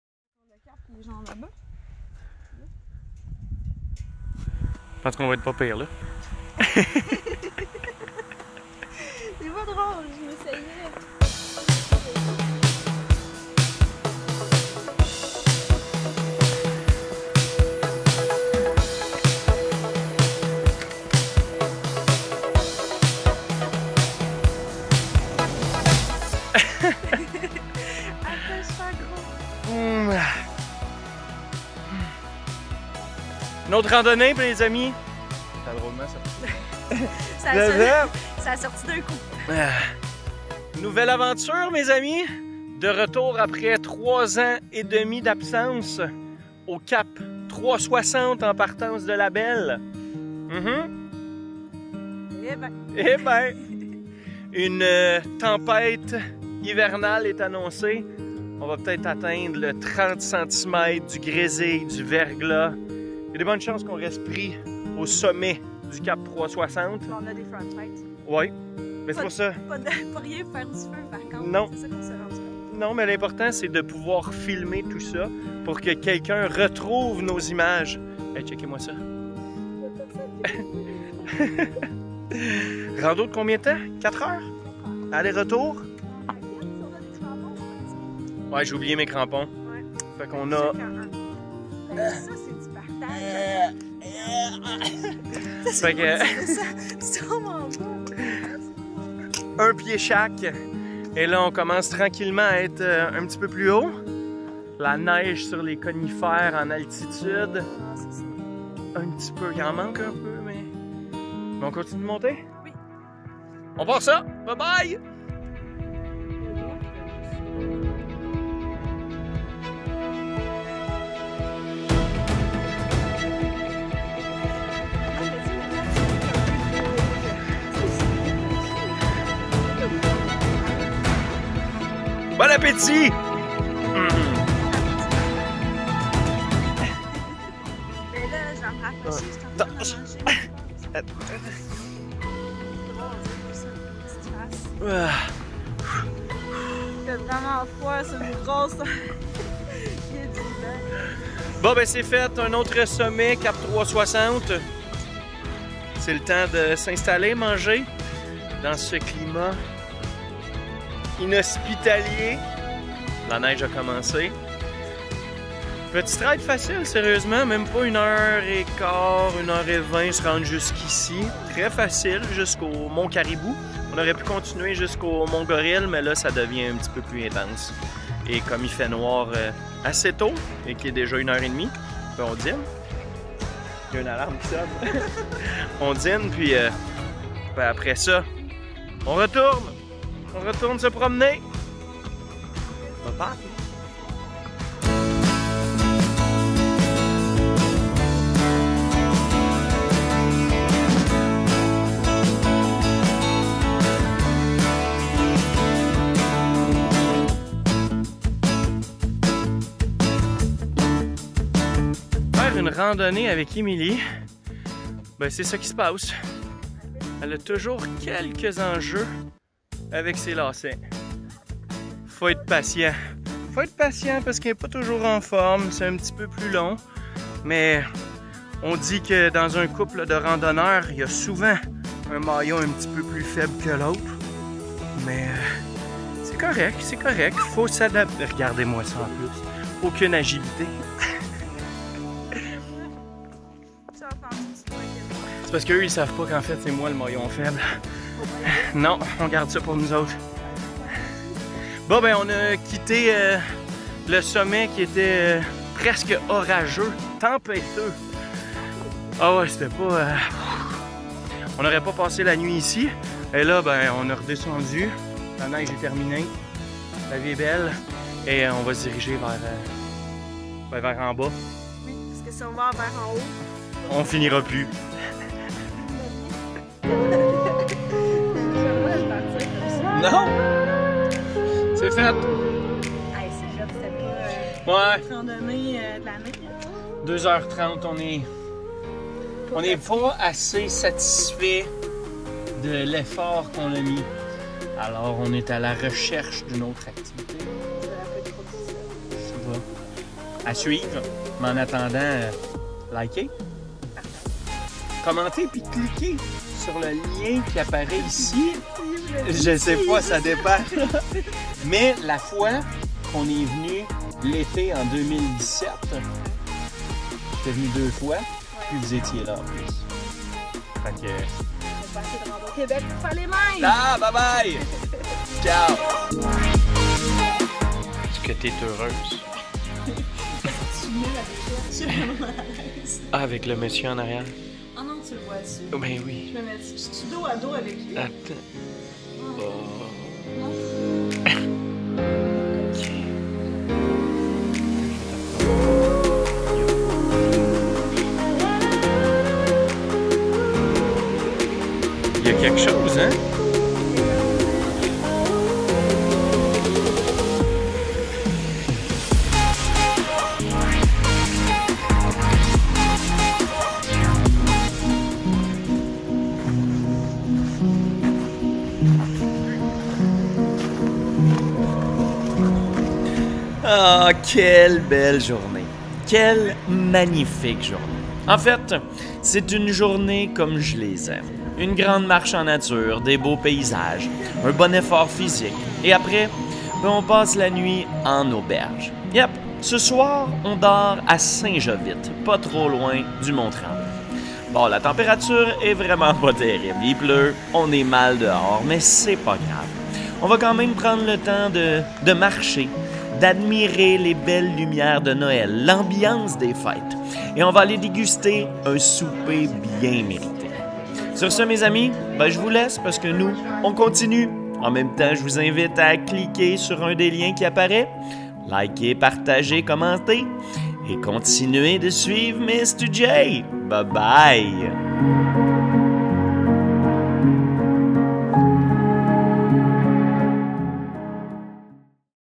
Québec Rando: Le sentier Cap 360! Le sommet du Mont Caribou, sur le chemin du Mont Gorille (2020)
Quelle belle journée.  Quelle magnifique journée.  En fait, c'est une journée comme je les aime.  Une grande marche en nature, des beaux paysages, un bon effort physique, et après, bien on passe la nuit en auberge. Yep, ce soir, on dort à St-Jovite.

Checkez ça!  On fait le sentier du Cap 360.  Et la prochaine fois, ce sera le Mont Gorille;)